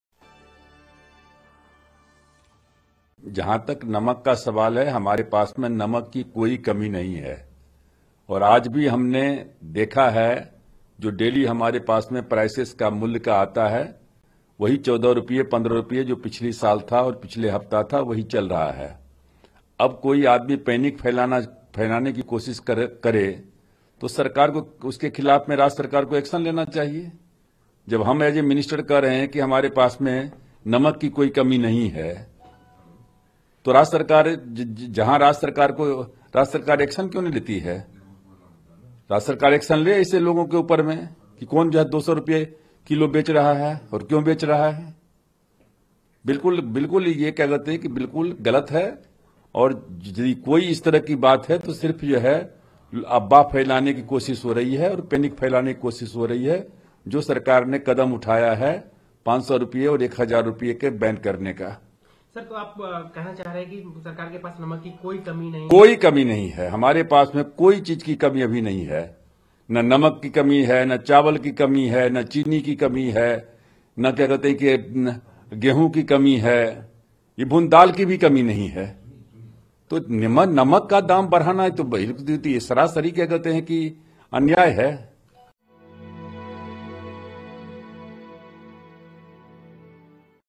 No Shortage Of Salt, Prices are Still Same: Ram Vilas Paswan
Union Minister of Consumer Affairs, Food and Public Distribution Ram Vilas Paswan on Saturday clarified the air by saying that there is no shortage of salt and the prices are still the same. He added that State Governments should take actions against the culprits who are spreading these rumours.